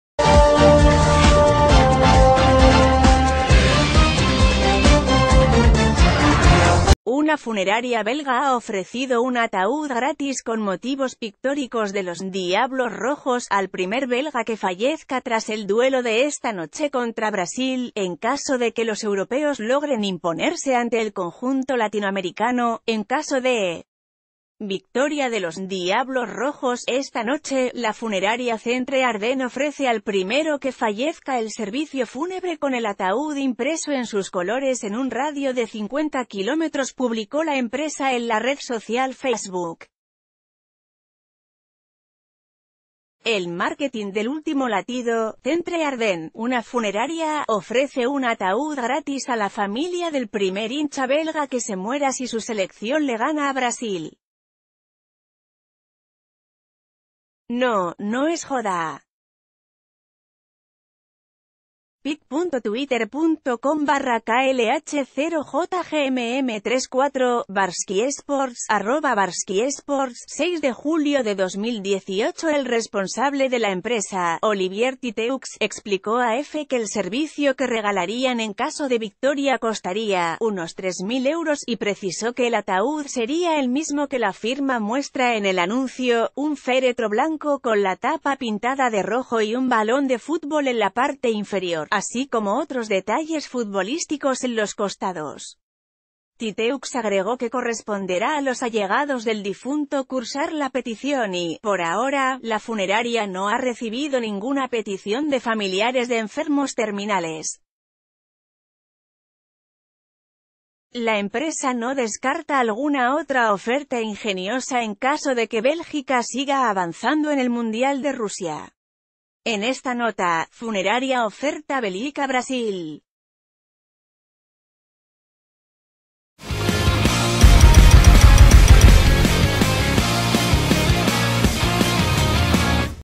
La loca oferta de una funeraria belga si Bélgica avanzaba
Una funeraria belga ha ofrecido un ataúd gratis con motivos pictóricos de los "Diablos Rojos" al primer belga que fallezca tras el duelo de esta noche contra Brasil, en caso de que los europeos logren imponerse ante el conjunto latinoamericano."En caso de victoria de los 'Diablos Rojos' esta noche, ...